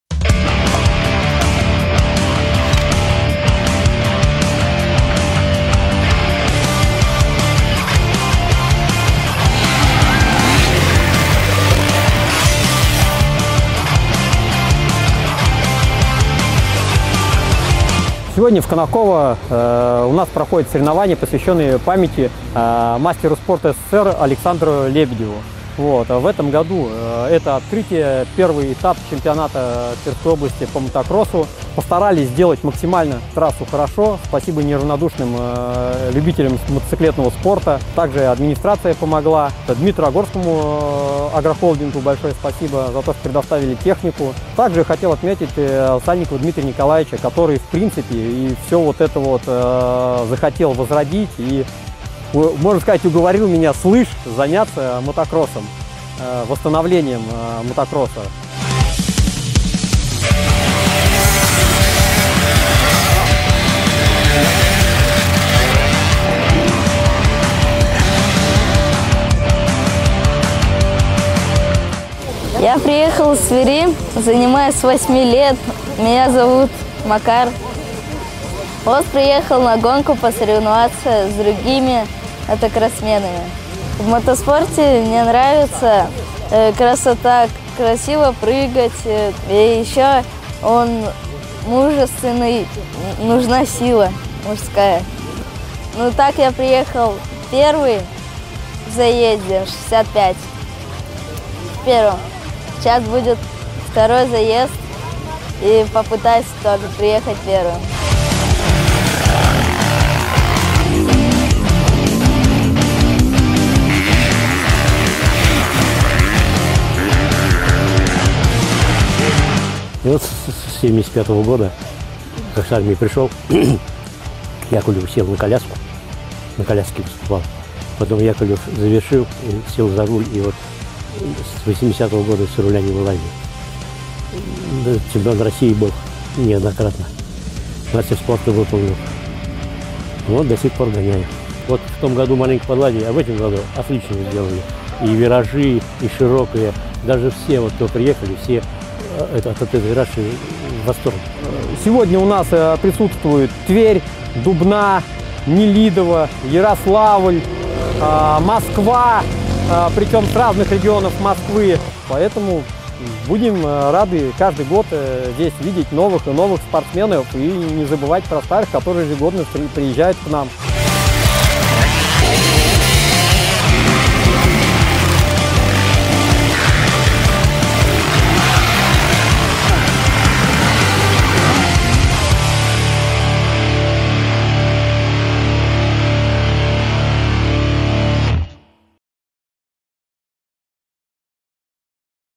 В Конаковском районе прошли соревнования по мотокроссу
На трассе близ села Селихово прошёл первый Этап Кубка Тверской области по мотокроссу памяти Мастера спорта СССР Александра Лебедева.

Участники соревнований высоко оценили подготовленную трассу, а также сам факт того, что на Конаковской земле возрождается мотокросс.

На трассе в этот день можно было встретить совсем юных спортсменов, а также ветеранов спорта. Конаковскому спортсмену Владимиру Данилову - 79 лет и он до сих пор активно участвует в соревнованиях.

Это был настоящий праздник спорта, который удался, благодаря отличной подготовке и помощи неравнодушных жителей!